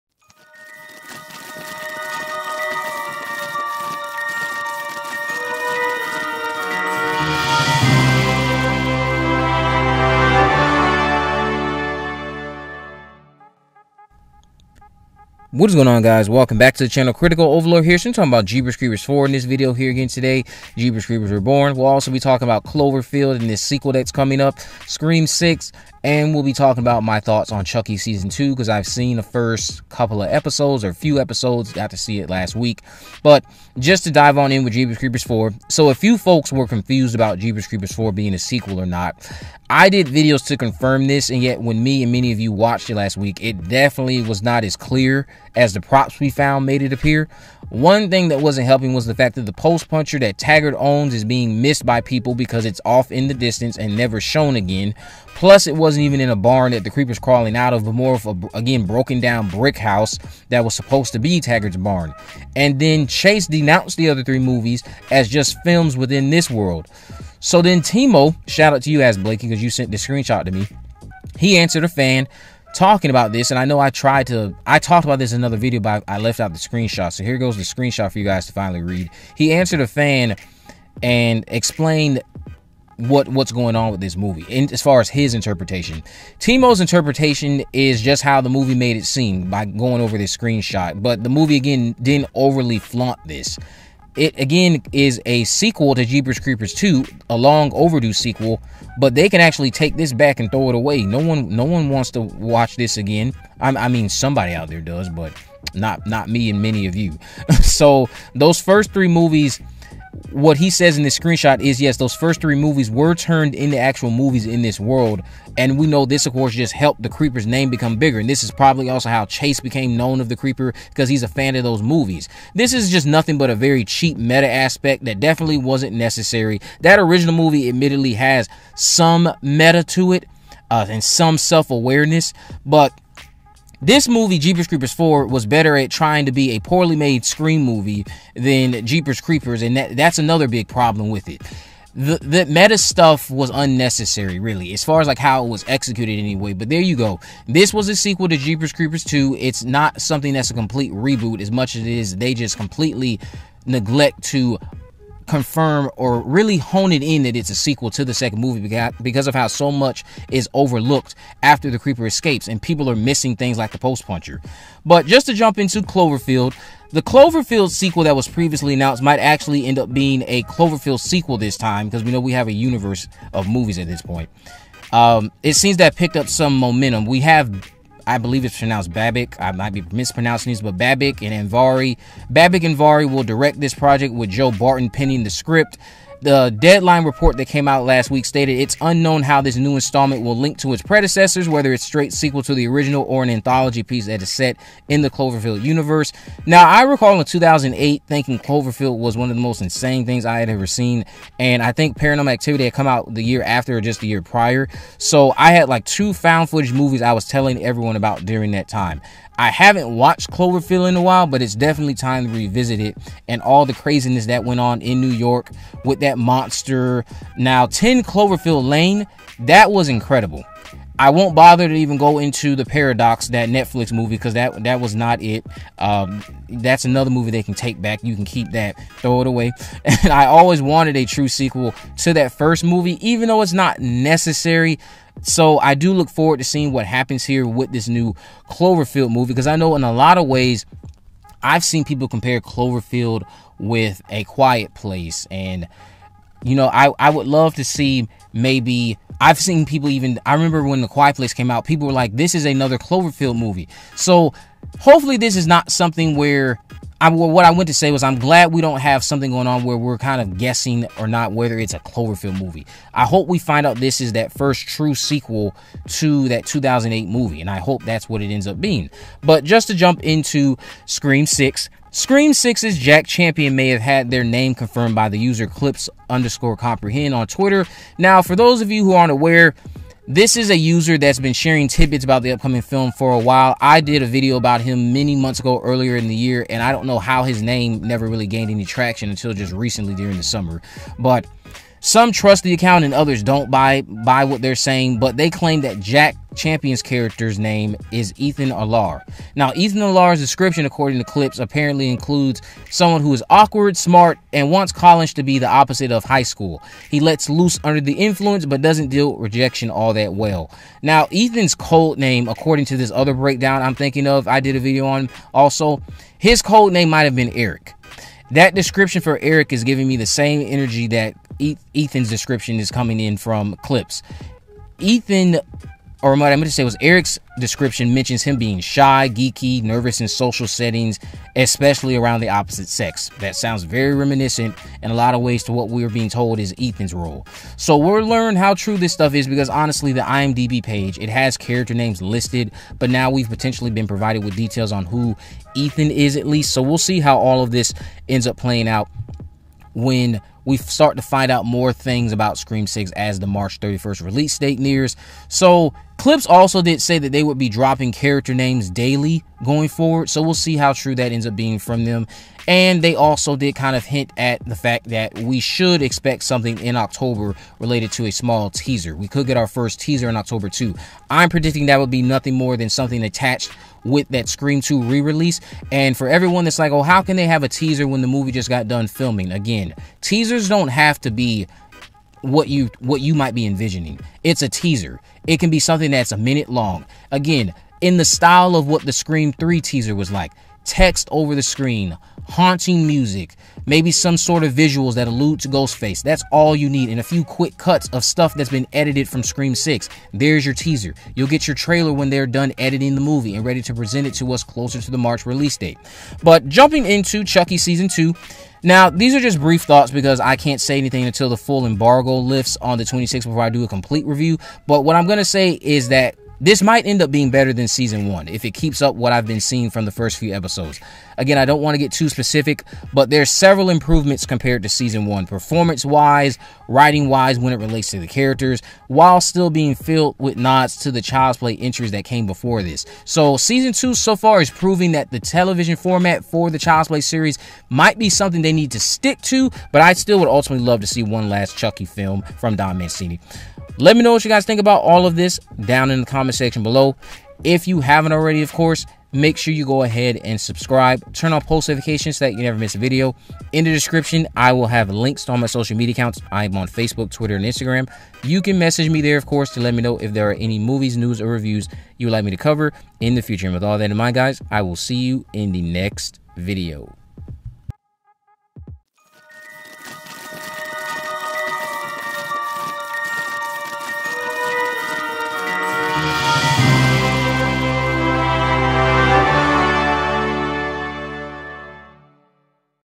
Jeepers Creepers 4 Update, Scream 6 Teaser Update, Cloverfield 2 NEWS, Chucky Season 2 Reaction
Jeepers Creepers 4 director has clarified if Jeepers Creepers: Reborn is a sequel after all. Cloverfield 2 has found its director and writer, but it's not known if this will be a true sequel to the 2008 Cloverfield movie. Scream 6 could showcase a first look at Ghostface New York killings in October and Jack Champions character details may have been revealed. Chucky Season 2 arrives next week but I've seen the first few episodes.

Art By: Samhain1992